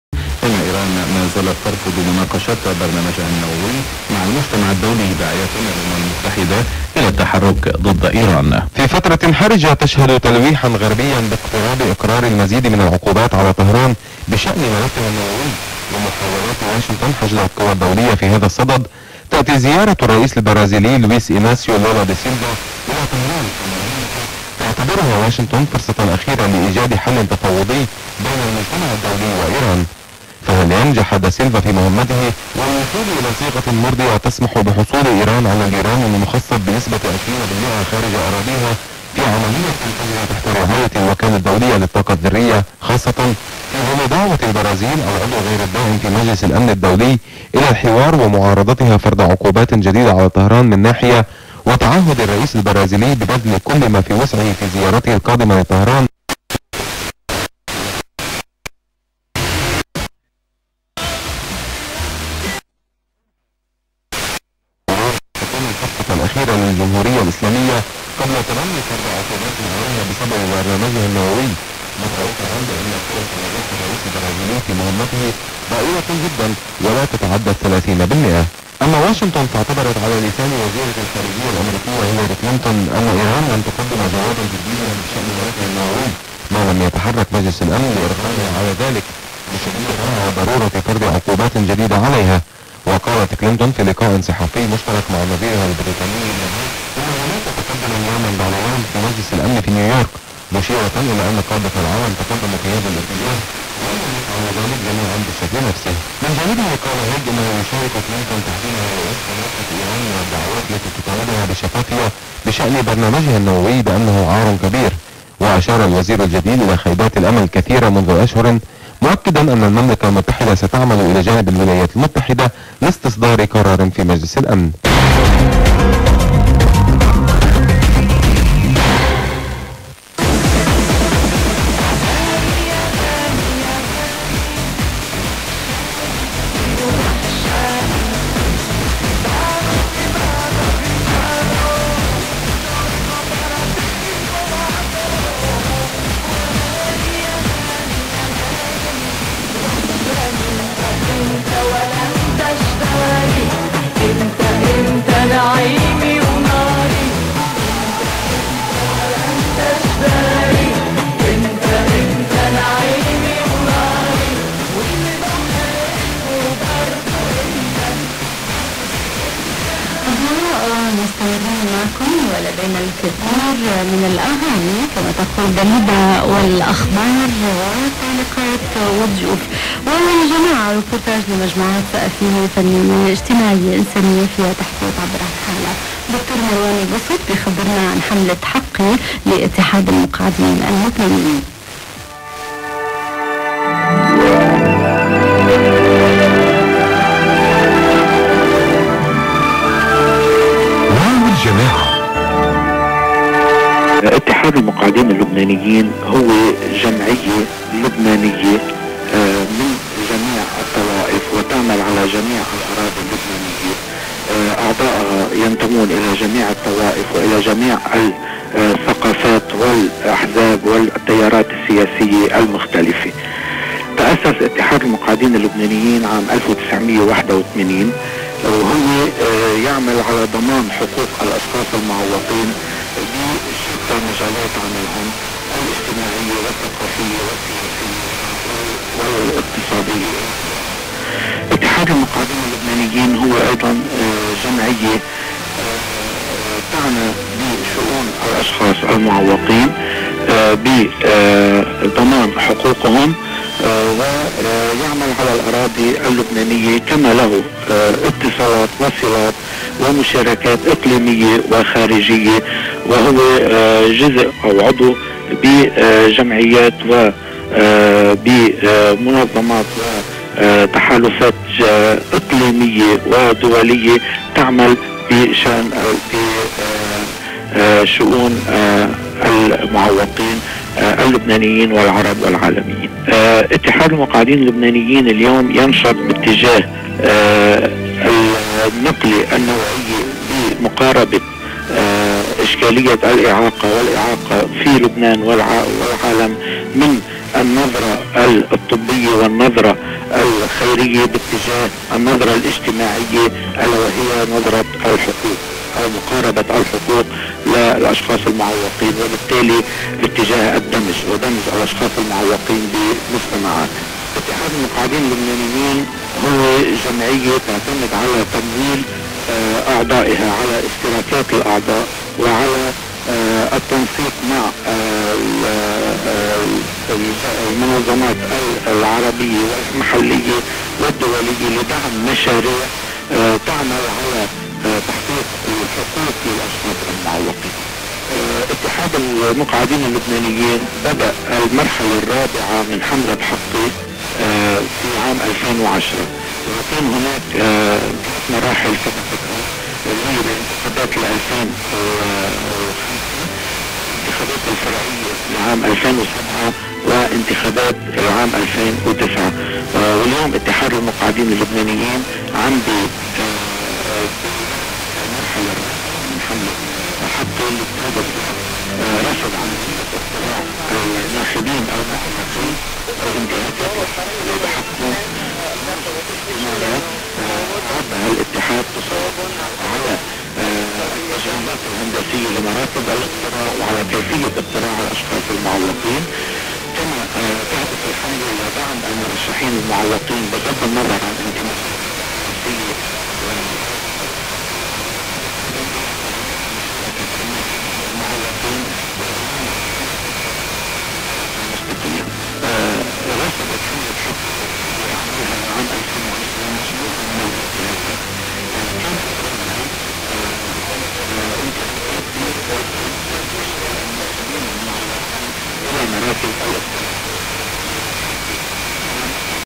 Radio Jamahiriya Libya via E-Skip on 88.3 MHz received in Hungary
14:06UTC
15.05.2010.
Location: Enying, Hungary
LJBC Radio Jamahiriya Benghazi relay station!